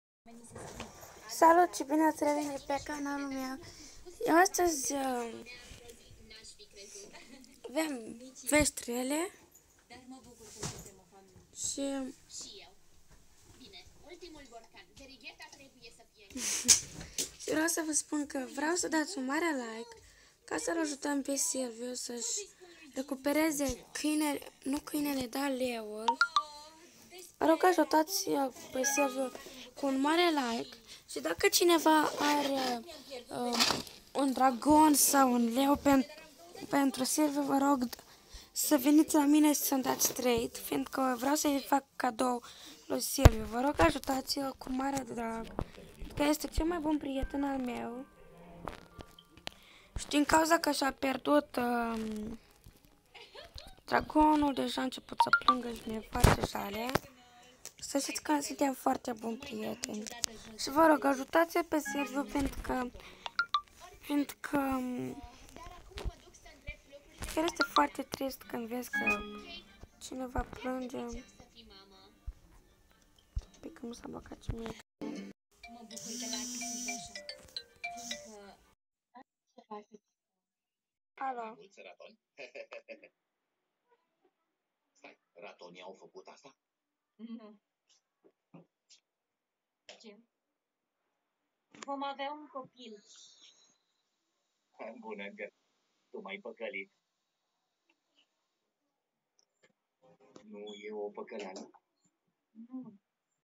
Va rog ajutați-l pe Silviu să-și găsească fericirea 😢
Va rog dacă cineva are un Pet neon, ori un dragon va dați-l mie și o să-l dau lui Silviu ssl fac fericit. Un Pet ori like un prieten fericit. Abonați-vă :
1)silviu is not a rebel
2)its razvan